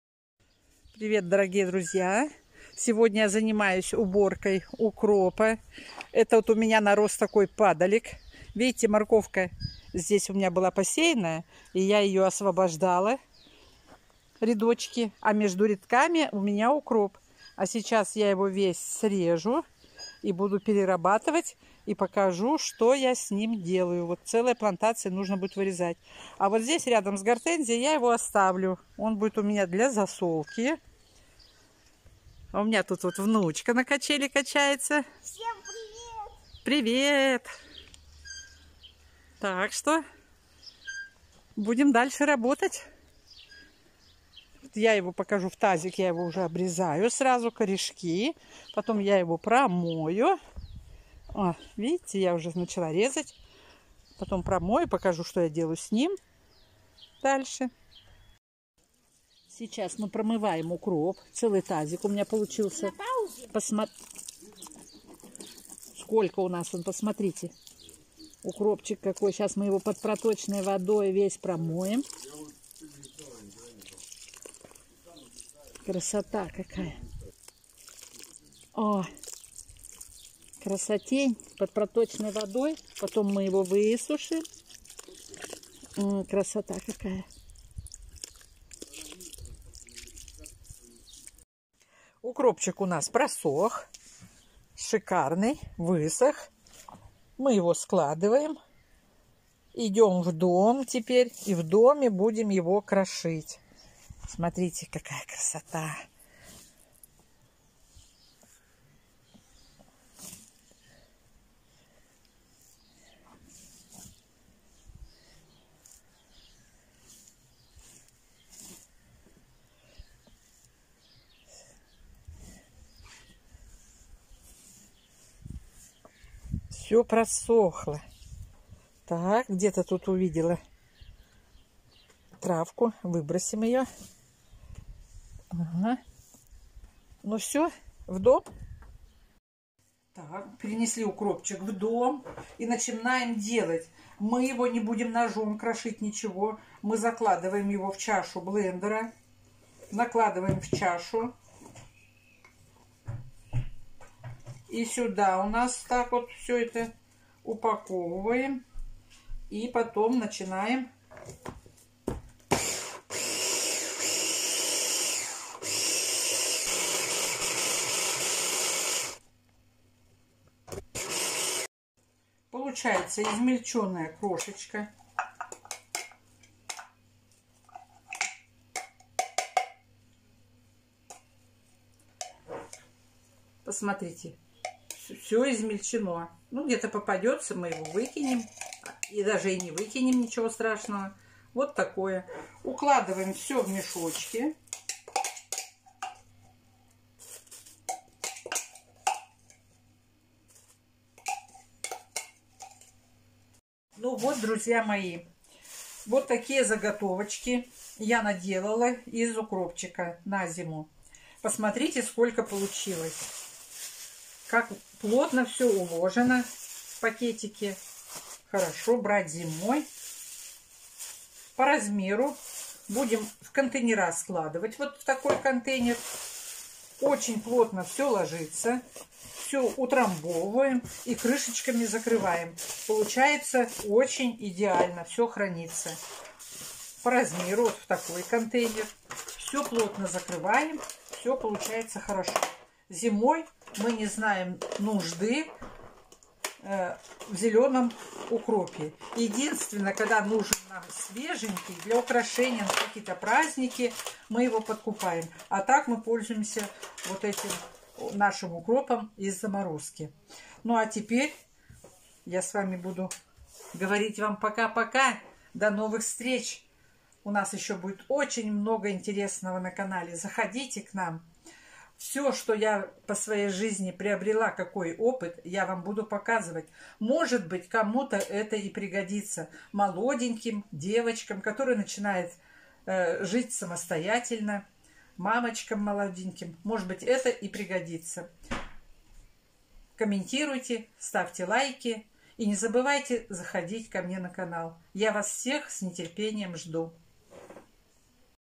Как заготовить зелёный укроп на зиму. Переработка и хранение, при полном сохранении полезных свойств
Подробно смотрите в видео как я это делаю, весь процесс от срезки до упаковки.